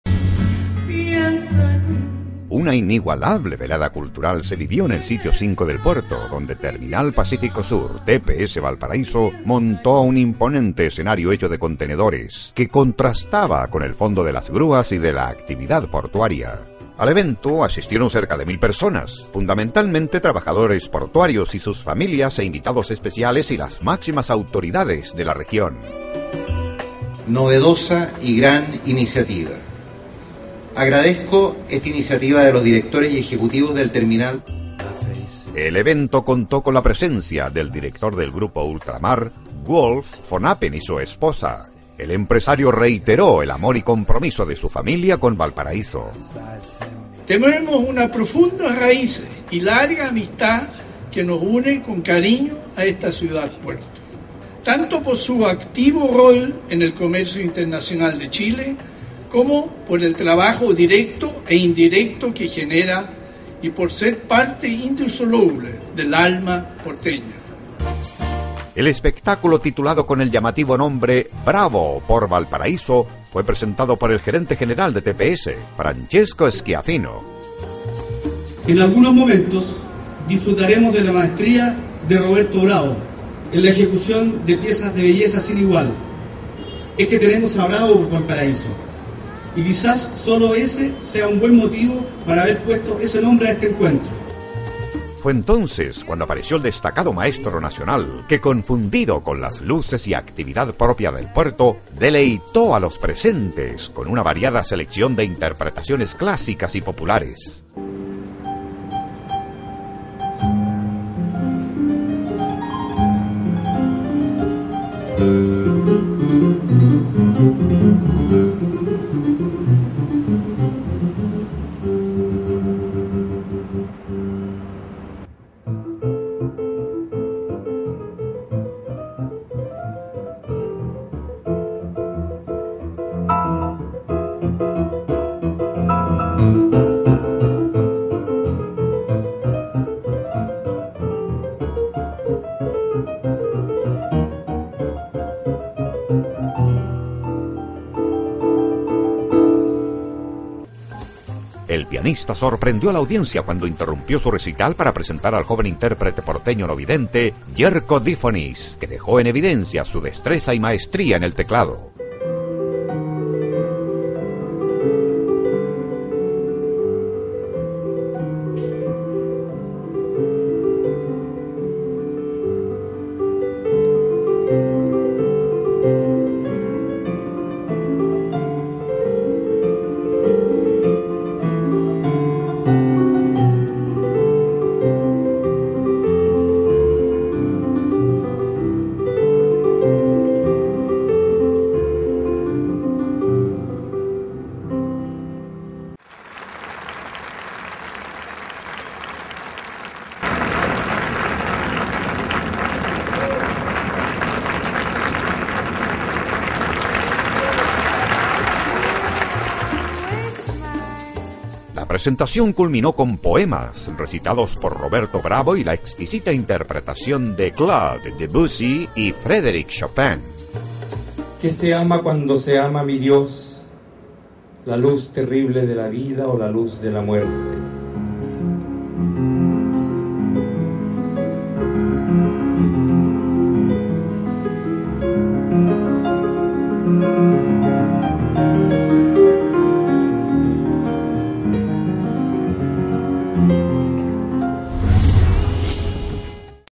Bravo Por Valparaíso
Completo reportaje a la presentación "Bravo por Valparaíso" del destacado pianista chileno Roberto Bravo en los recintos portuarios de Terminal Pacífico Sur, TPS Valparaíso, donde asistió el líder del grupo Ultramar, Wolf von Appen, ejecutivos y trabajadores del puerto y autoridades regionales de Valparaíso.